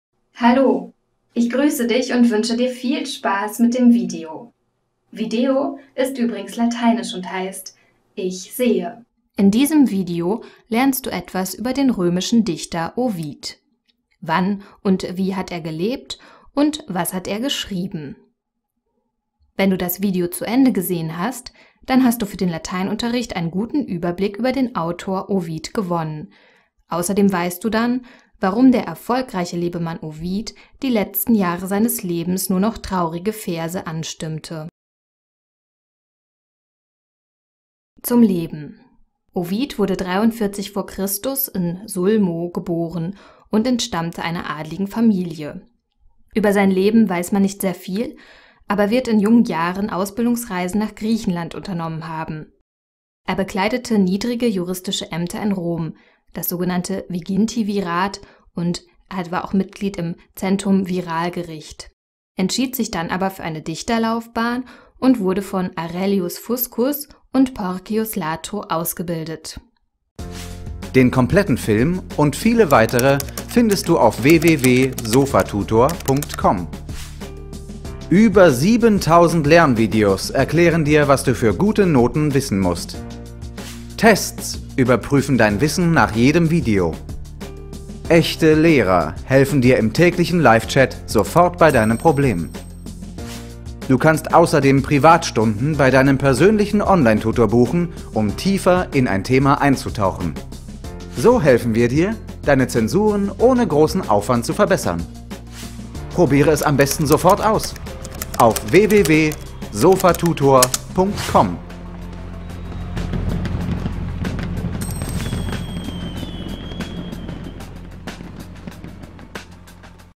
Leben und Werk von Ovid | Latein | Autoren und Werke
In diesem Video beschäftigen wir uns mit Leben und Werk des römischen Dichters Publius Ovidius Naso (Ovid). Dabei erfährst du wichtige Eckdaten und Details. Auch auf seine Werke Ars Amatoria und Metamorphosen werden wir genauer eingehen, da diese für den Lateinunterricht sehr relevant sind.

Dieses Video gehört zu: Latein | Autoren und Werke | Dichtung